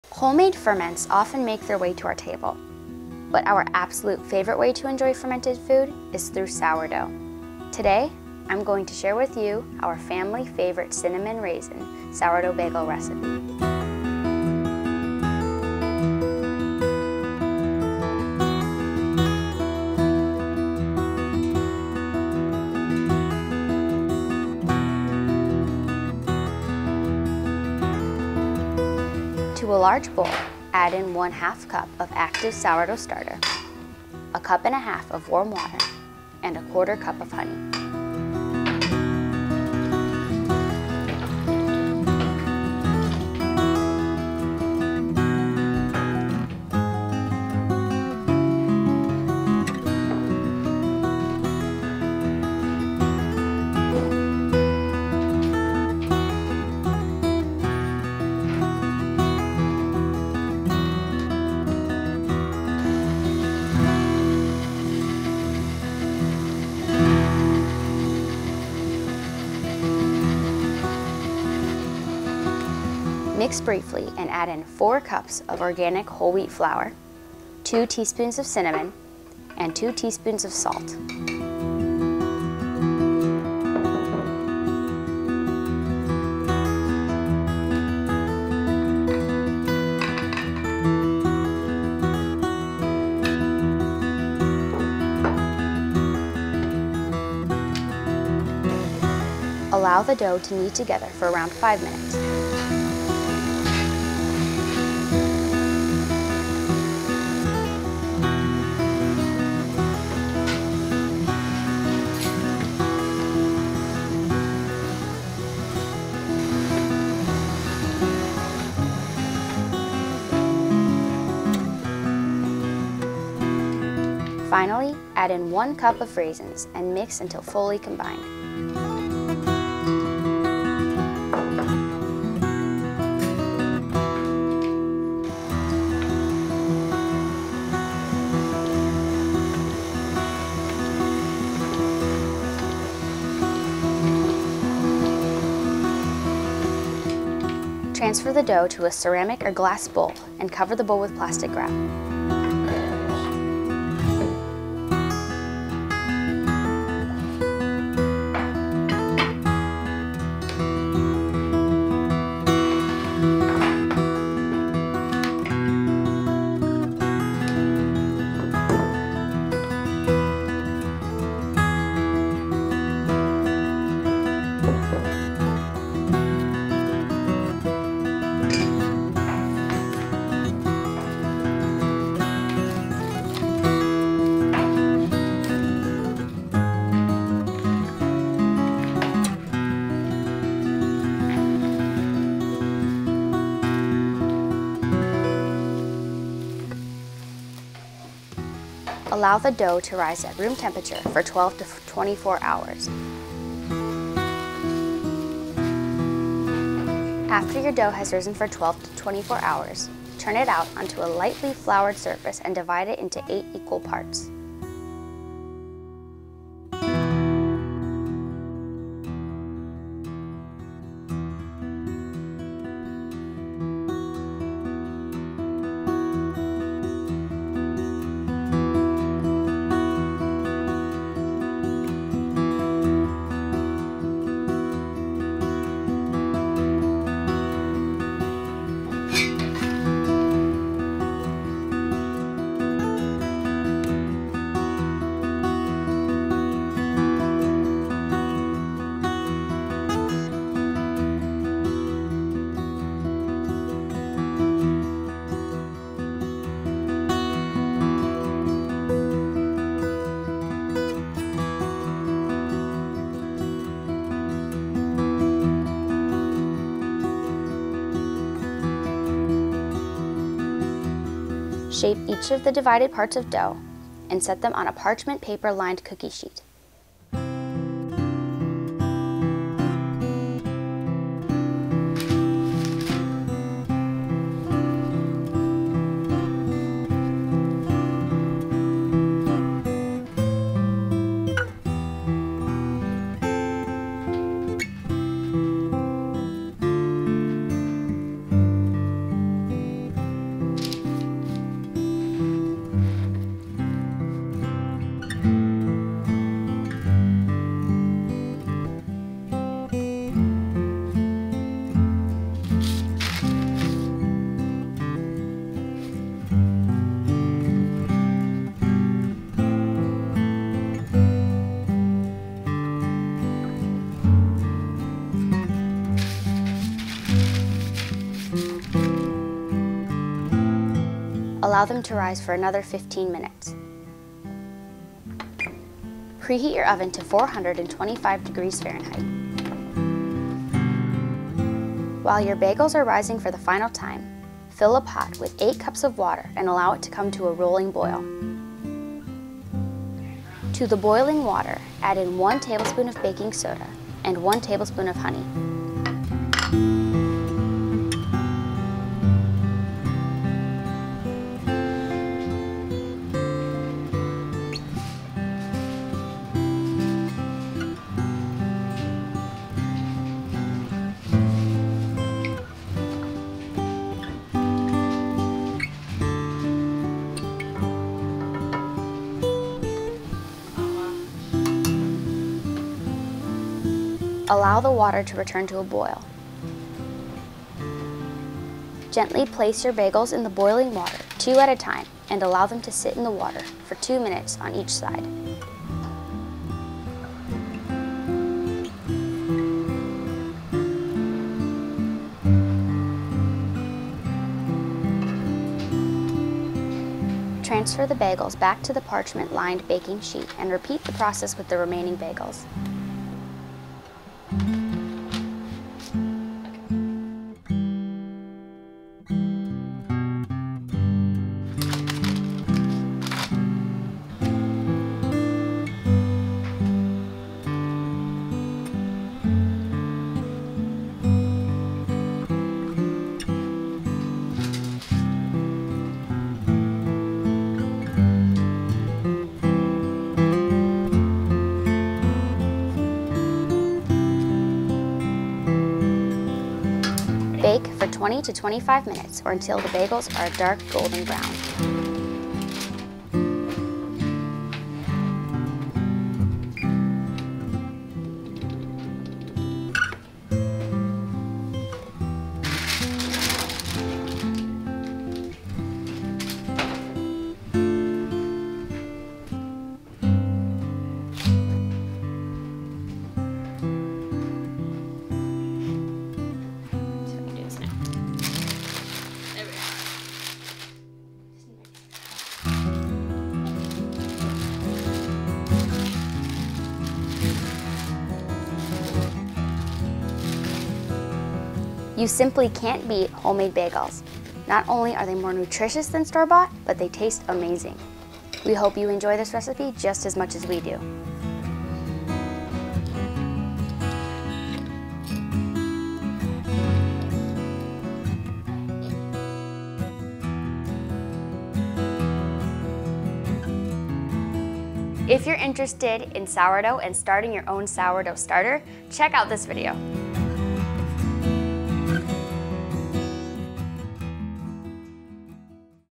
Easy Bagel Recipe | Homemade Sourdough Cinnamon Raisin | Fermented February | Fermented Foods
Julianne shares her recipe for delicious homemade sourdough bagels. Jules has been exploring creative ways to use her fermented sourdough starter, and this eeay bagel recipe is one of our favorites so far! In this step-by-step video, she'll show you how to make amazing bagels at home.

Cinnamon Raisin Sourdough Bagels:

1/2 cup sourdough starter
1 1/2 warm water
1/4 cup honey
4 cups organic whole wheat flour
2 teaspoons ground cinnamon
2 teaspoons salt
1 cup raisins

For Water Bath:
8 cups water
1 tablespoon baking soda
1 tablespoon honey

To a large bowl add in 1/2 cup of active sourdough starter, 1 1/2 cup of warm water, and 1/4 cup of honey. Mix briefly and add in 4 cups of organic whole wheat flour, 2 teaspoons of cinnamon, and 2 teaspoons of salt. Allow the dough to kneed together for around 5 minutes. Finally, add in 1 cup of raisins and mix until fully combined. Transfer the dough to a ceramic or glass bowl and cover the bowl with plastic wrap. Allow the dough to rise at room temperature for 12 to 24 hours.

After your dough has risen for 12 to 24 hours turn it out onto a lightly floured surface and divide it into eight equal parts. Shape each of the divided parts of dough and set them on a parchment paper lined cookie sheet. Allow them to rise for another 15 minutes.

Preheat your oven to 425 degrees f.

While your bagels are rising for the final time, fill a pot with 8 cups of water and allow it to come to a rolling boil. To the water add in 1 tablespoon of baking soda and 1 tablespoon of honey. Allow the water to return to a boil. Gently place your bagels in the water two at a time and allow them to sit in the water for 2 minutes on each side. Transfer the bagels back to the parchment lined baking sheet and repeat the process with the remaining bagels. Bake for 20 to 25 minutes or until the bagels are a dark golden brown. Enjoy!

Amazon Store: